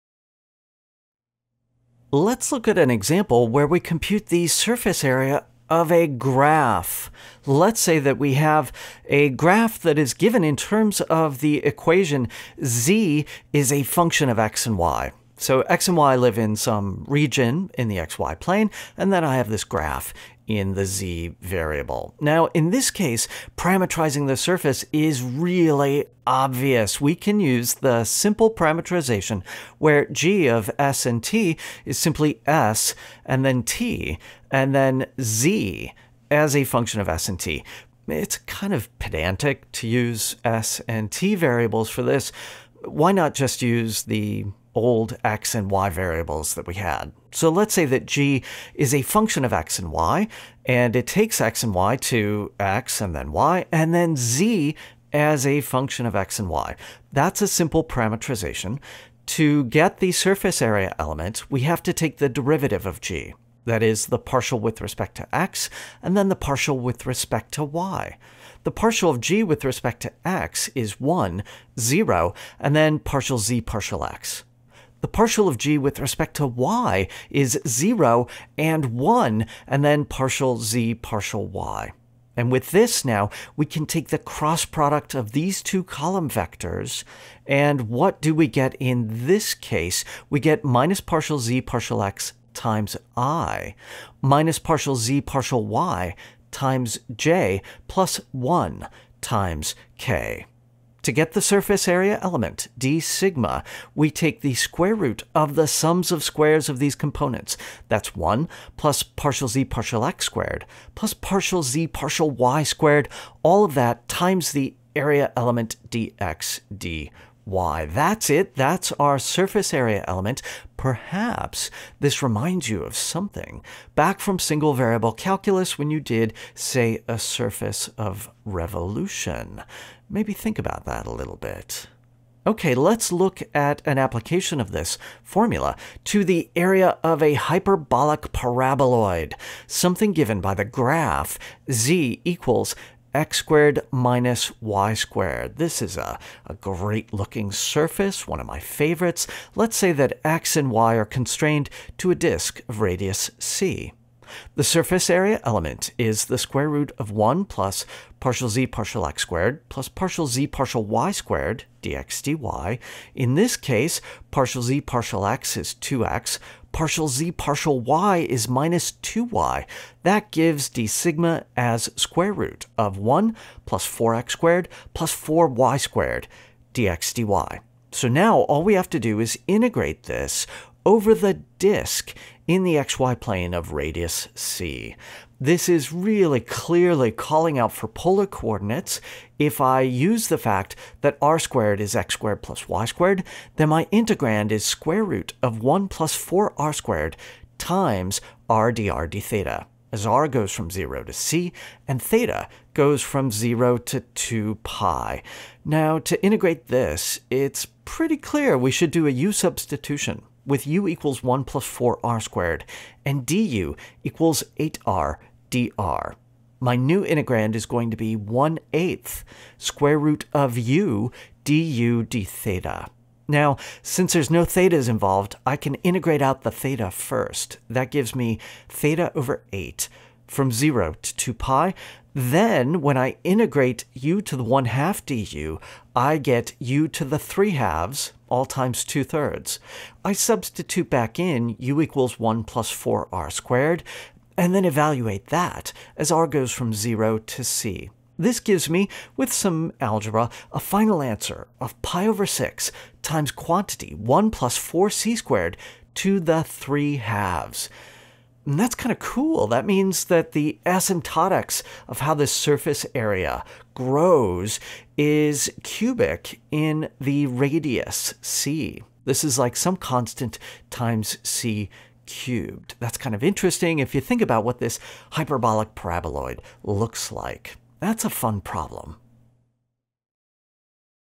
CalcBLUE 3 : Ch. 17.3 : Surface Area of a Graph
Let's apply what we've learned about the surface area element to the case of a surface represented as the graph of a function.